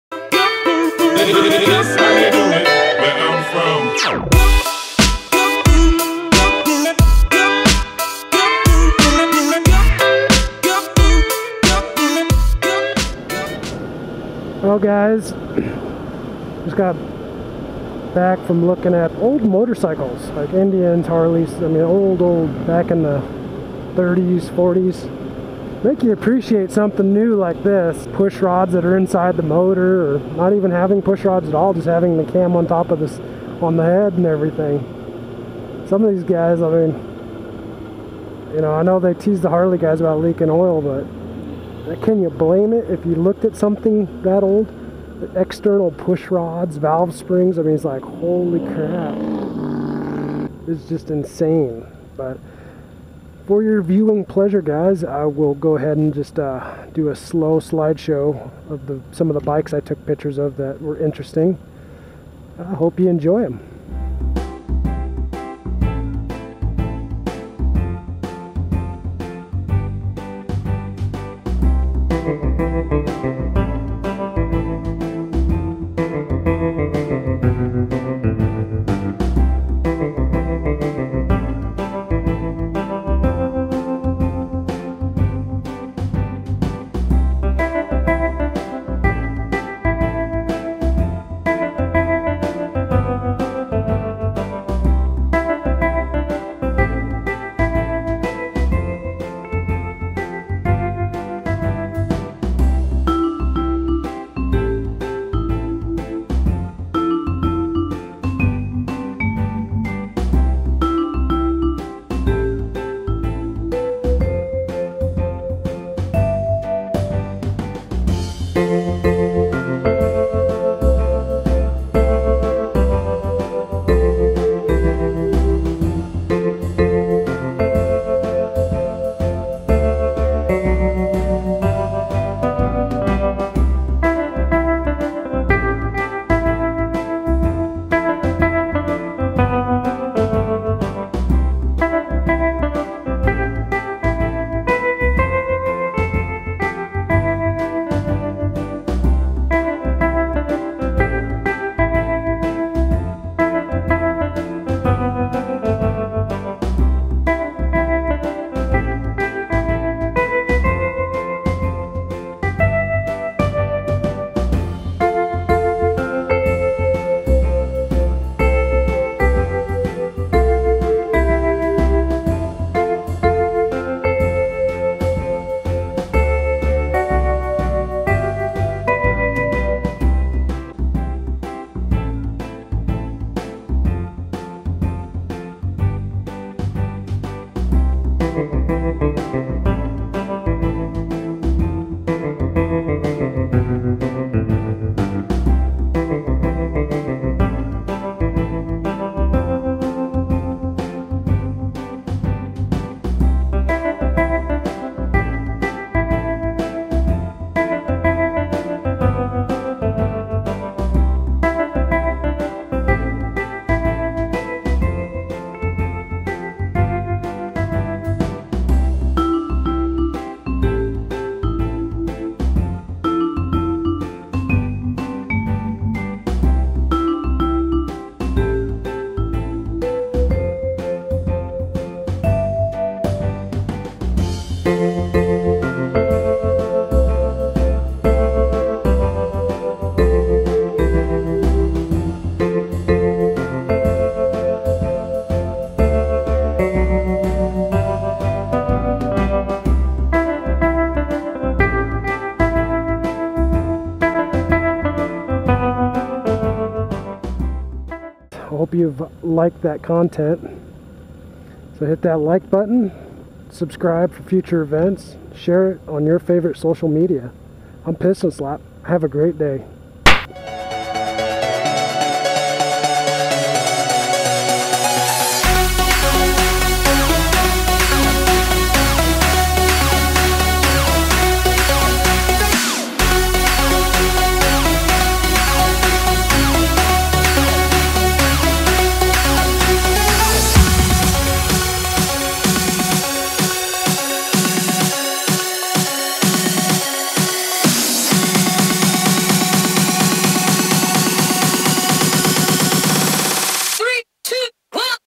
Old bike show
In this episode I visit a vintage motor cycle show.

All speeds displayed and sounds of engines racing have been digitally enhanced for your viewing pleasure, Video is 1 or more years old stock vide